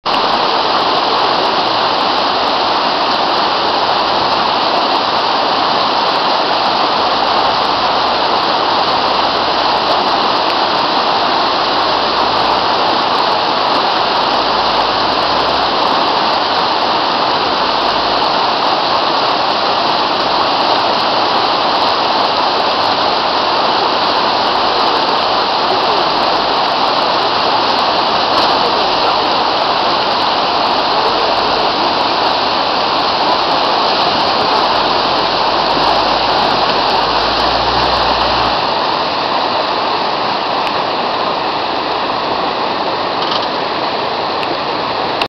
Mitsubishi Pajero (Montero) crossing a Kensu river ford in Kazakhstan off-road -2
This is one sturdy and robust SUV. Very good in all off-road, mud & mire, trail, and contain action